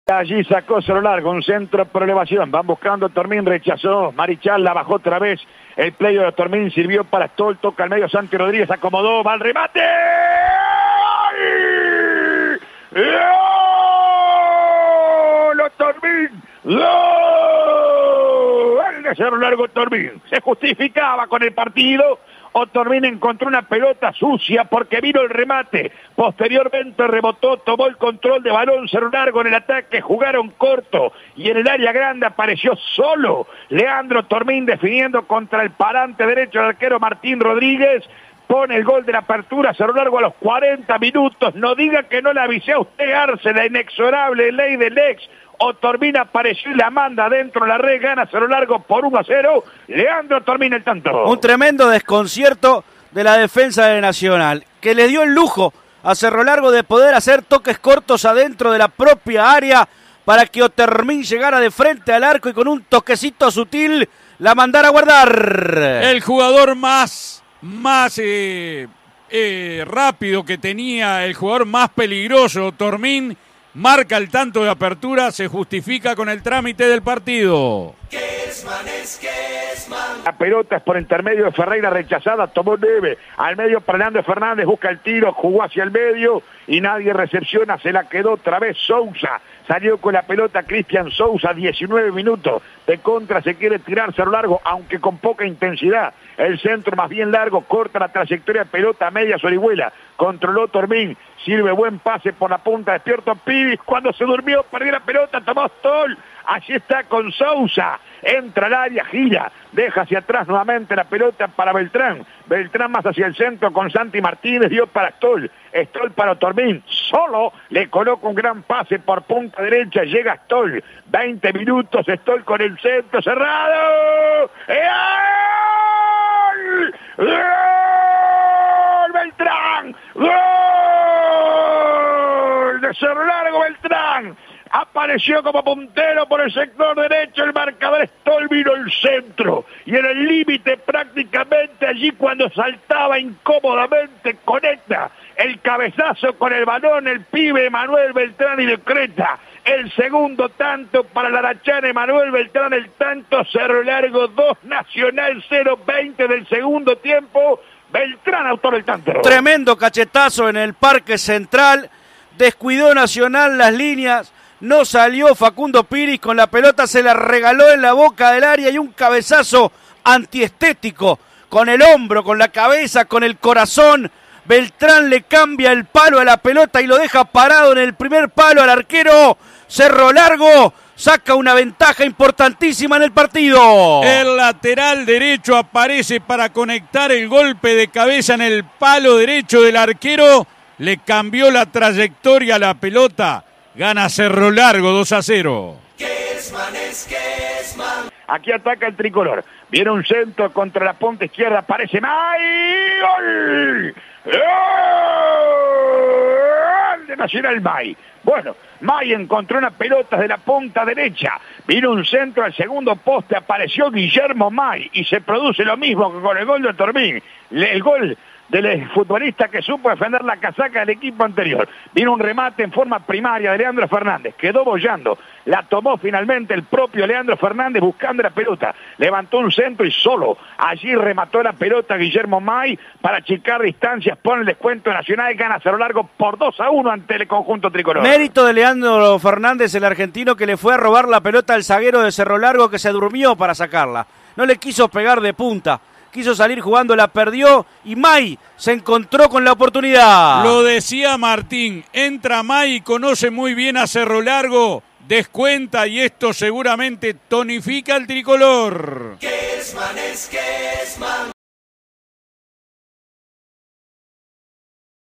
Nacional 1 Cerro Largo FC 2 - Apertura 2021 (por Martín Kesman)
Los goles del partido, por Martín Kesman, Richard Arce y Ernesto Faría para CX22 Radio Universal 970 AM.